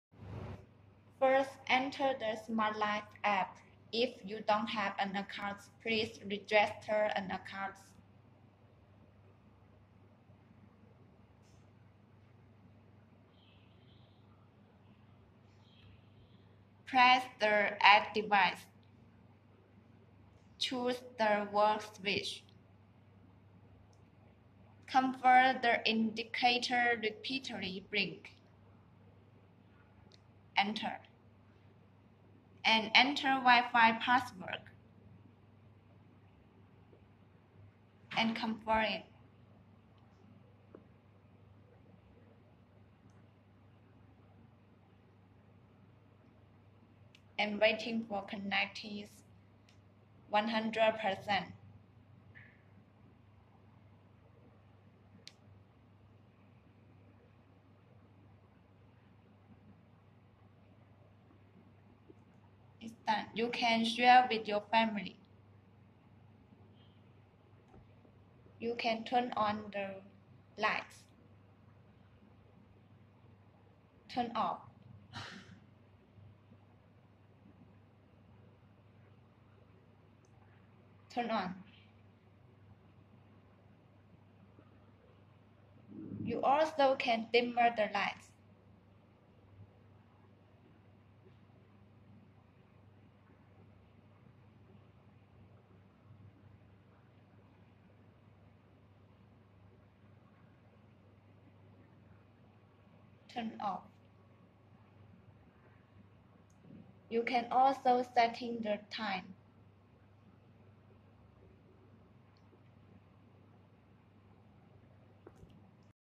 Wifi wall dimmer switch connections
This is the demo for dimmer wall switch. You can app control by your phone,you can also voice  control by echo